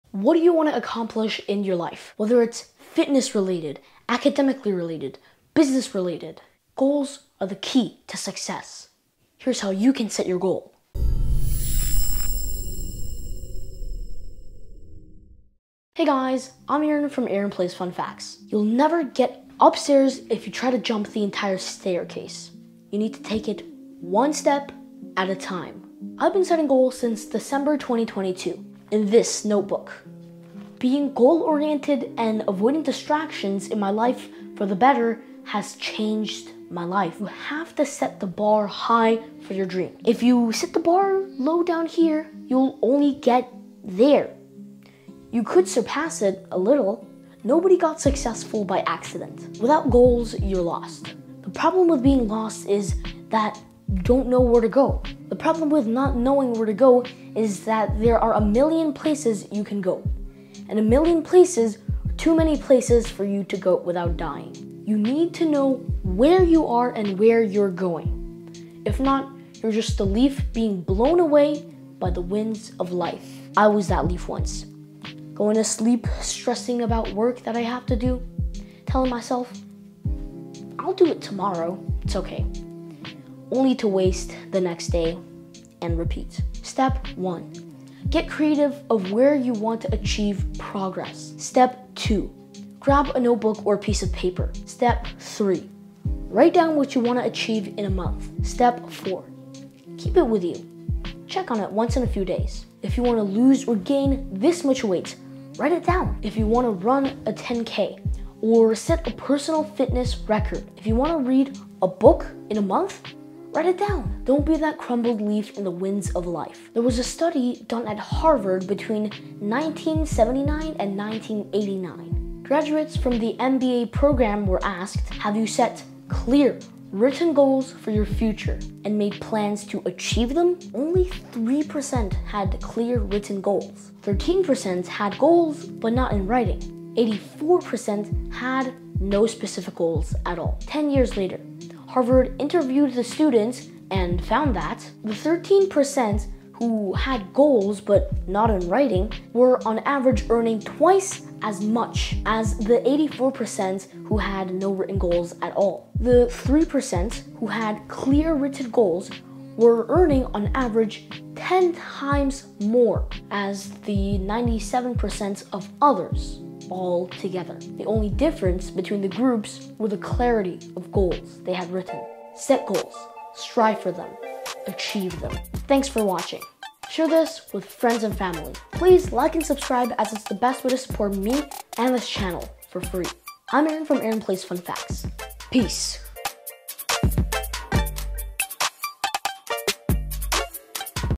Why You Should Set Goals | Aaron Wang Experience
What do you want to accomplish in your life?
Whether it's fitness-related, academically related, business-related;
Goals are the key to success.
Here's how you can set your goal.

You'll never get upstairs if you try to jump the entire staircase.
You need to take it one step at a time.
I've been setting goals from December 2022 in this notebook.
Being goal-oriented and avoiding distractions in my life for the better has changed my life.
You have to set the bar high for your dreams.
You will only get there if you set the bar low down here.
You could surpass it a little.
Nobody got successful by accident.
Without goals, you're lost.
The problem with being lost is that you don't know where to go.
The problem with not knowing where to go is that there are a million places you could go.
And a million places are too many for you to go without dying.
You need to know where you are and where you're going.
If not, you're just a leaf being blown away by the winds of life.
I was that leaf once.
Going to sleep stressing about work that I have to do.
Telling myself I'll do it tomorrow, it’s okay, only to waste the next day and repeat.

Here's how to set goals.
Step 1: Get creative on where you want to achieve progress.
Step 2: Grab a notebook or a piece of paper.
Step 3: Write down what you want to achieve in a month.
Step 4: Keep it with you, and check on it once in a few days.

If you want to lose or gain this much weight, write it down.
If you want to run a 10k, or set a personal fitness record, if you want to read a book a month, write it down.
Don't be that crumbled leaf in the winds of life.

There was a study done at Harvard between 1979 and 1989. Graduates from the MBA program were asked “Have you set clear written goals for your future and made plans to achieve them?” The results of that question were:
• Only 3% had written goals and plans
• 13% had goals but not in writing
• 84% had no specific goals at all
10 years later Harvard interviewed the students and found that:
1. The 13% who had goals but not in writing were on average earning twice as much as the 84% of those who had no written goals at all
2. The 3% who had clear, written goals were earning on average 10 times as much as the 97% of others altogether. The only difference between the groups is the clarity of the goals they had written
Set goals, strive for them, and achieve them.

Hey guys, I’m Aaron. I love to explore and share amazing facts about our world! Hope you can join this big family and just have some fun learning! Thanks, and love you guys!